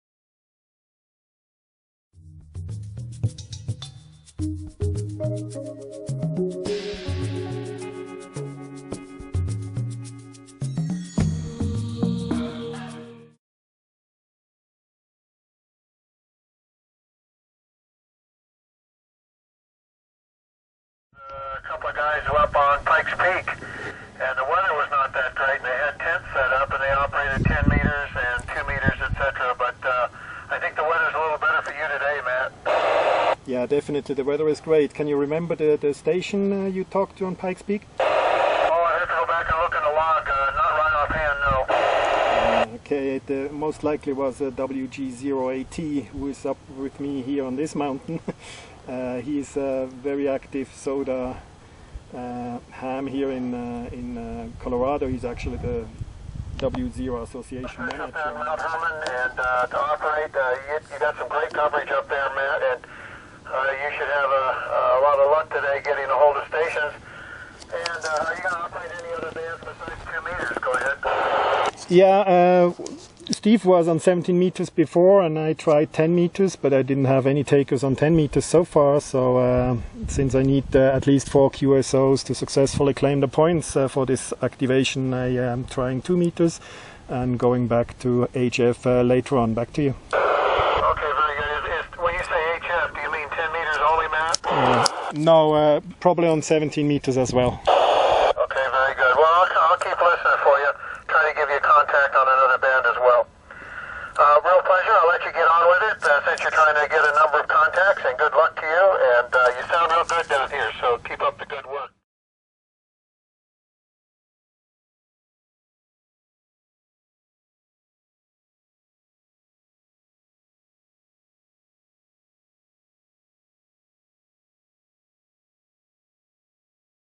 Ultralight 3-el. Yagi for 2m
Ultralight home-brew 3-element Yagi in action on Mount Herman CO